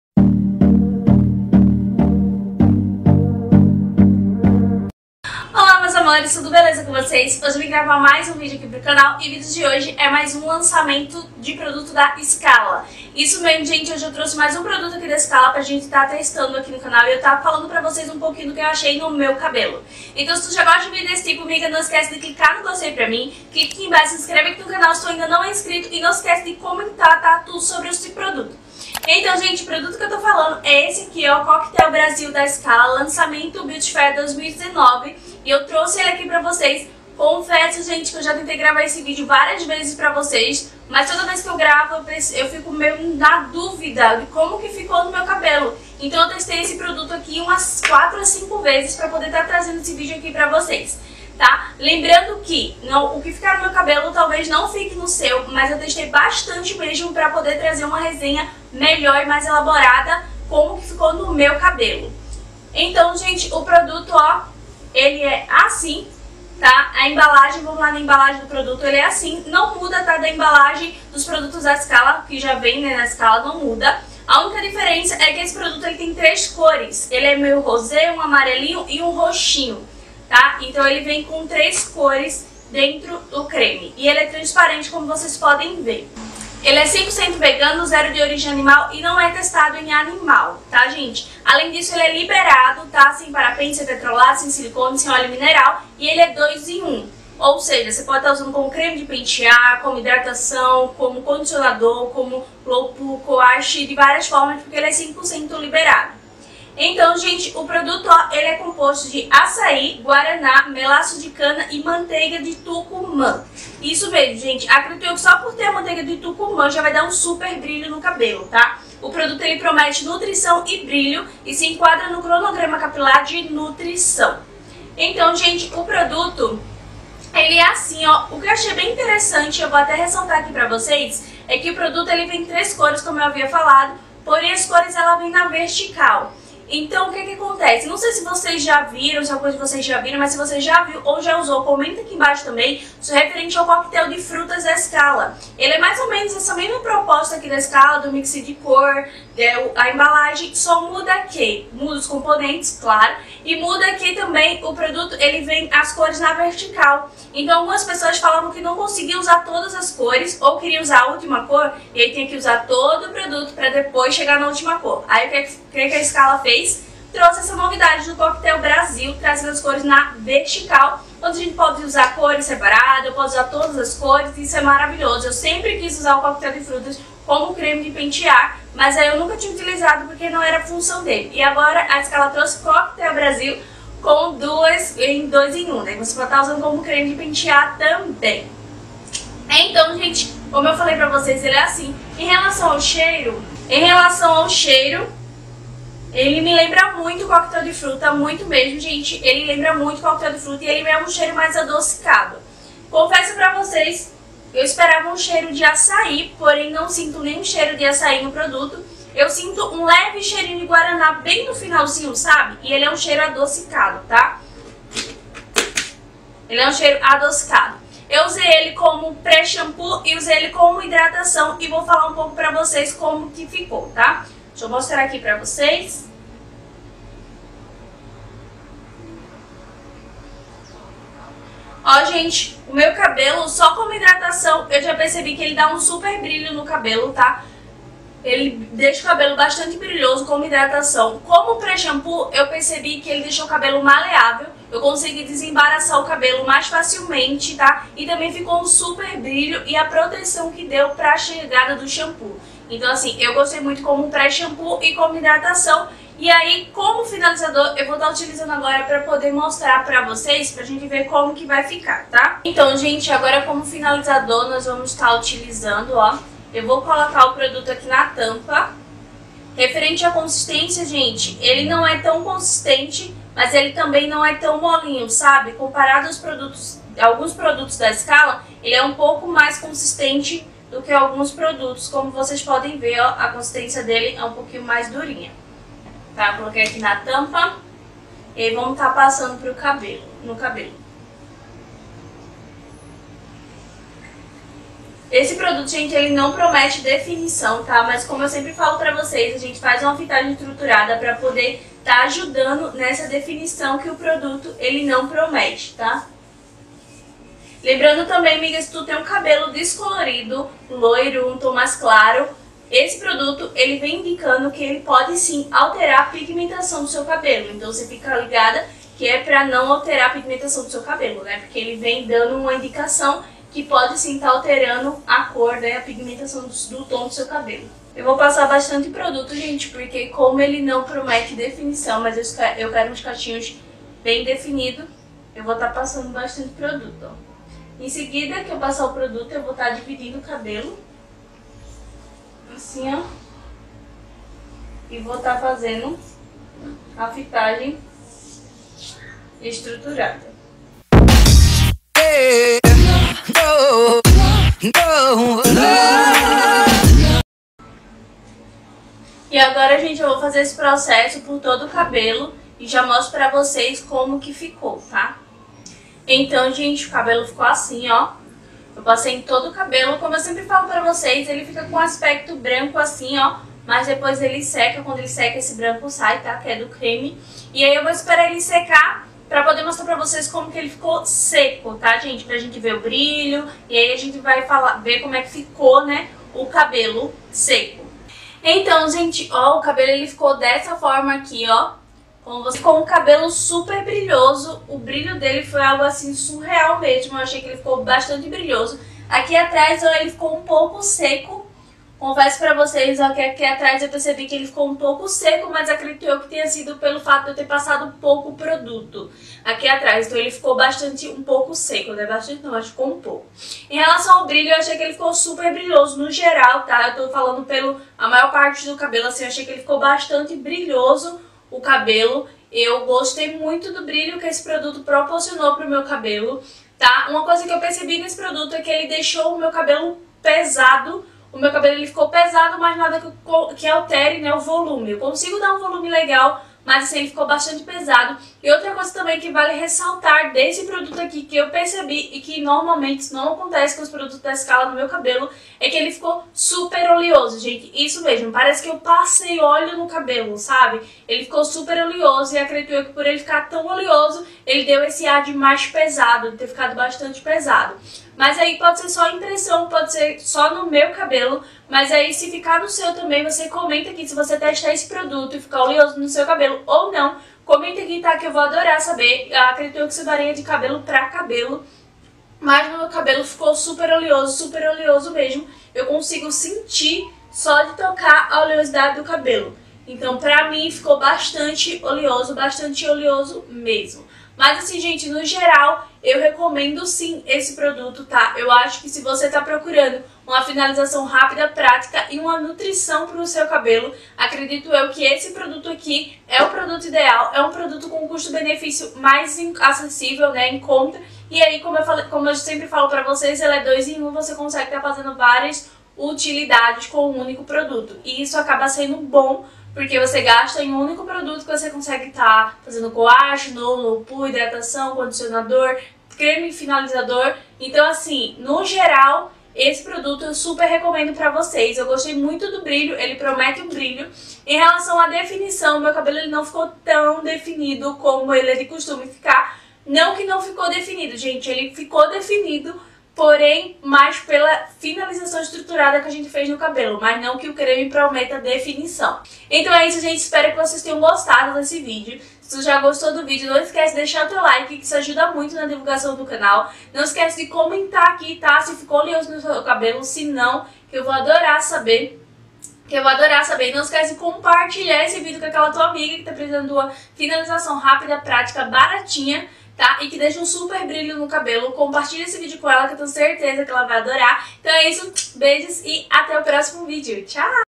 FINALIZAÇÃO COM O CREME SKALA COQUETEL BRASIL( Vegano/ No/ Low Poo) -LANÇAMENTO
Olá meus amores, no vídeo de hoje trago mais um lançamento da SKALA COQUETE BRASIL, finalizei o cabelo , testei como
pré-shampo, hidratação e contei tudo para vocês , espero que gostem!!

criei um canal só sobre dicas de casa, faxina , dicas de como viver no aluguel, comprinhas para casa e muito mais .